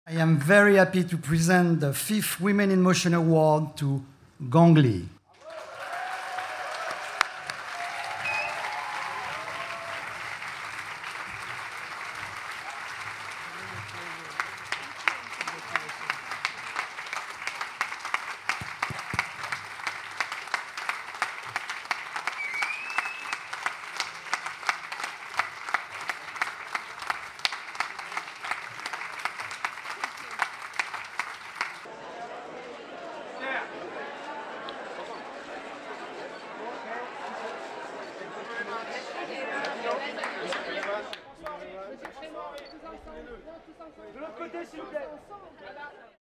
Gong Li honored with Women in Motion award
Actress Gong Li honored with the Women in Motion award at the Cannes Film Festival. (May 20)